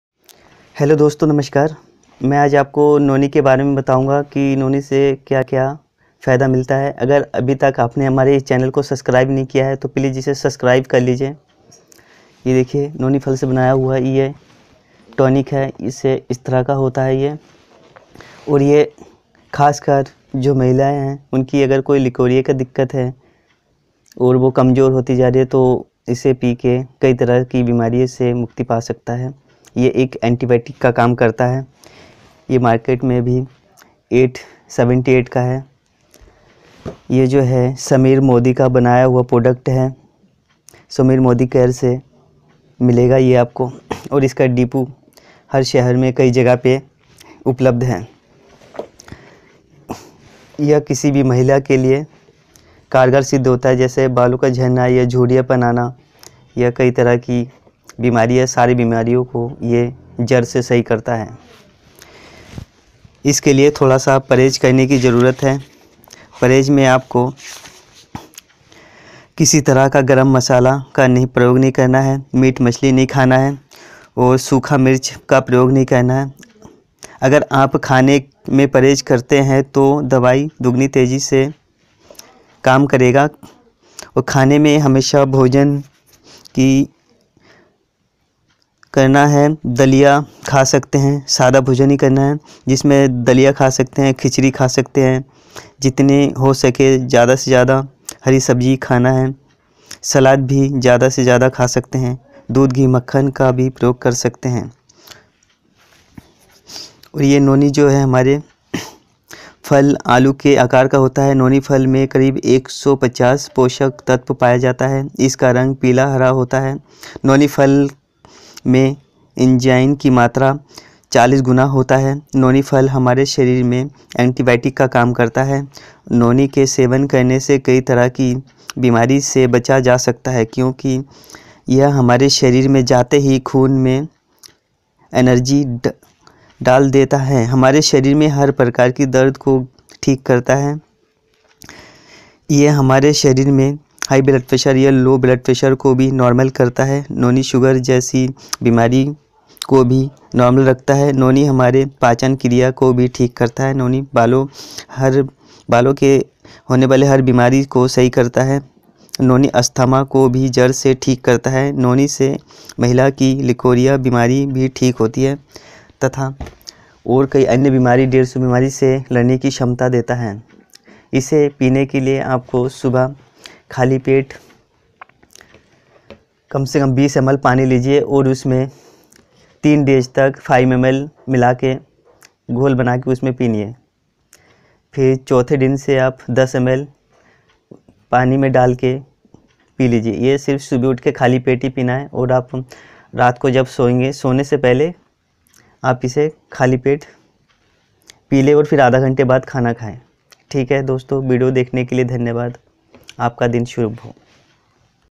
How to Noni Juice Concentrate in hindi-शरीर के लिए संजीबनी बूटी है
Noni Juice Concentrate Enriched wlth KOKUM .                                                                   Directions for Use; Drink on empty stomach , 30 minutes before meal. can be taken as is or diluted with water or as directed by the physician.100% herbal ,